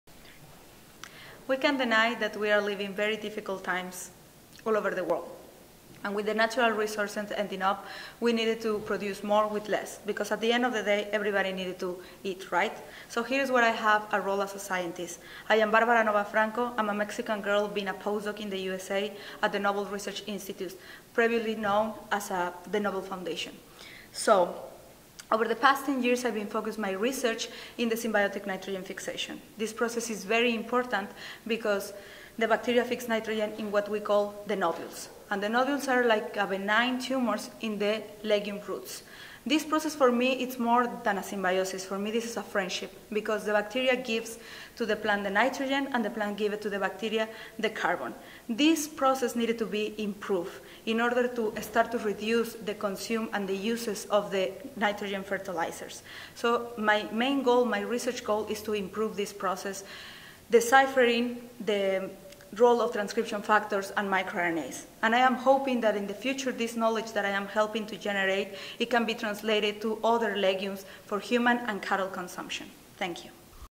PB17 Pitch Your Science - Barbara Nova-Franco
Barbara Nova-Franco presents a short recap of her plant biology research at ASPB 2017 in Honolulu, HI.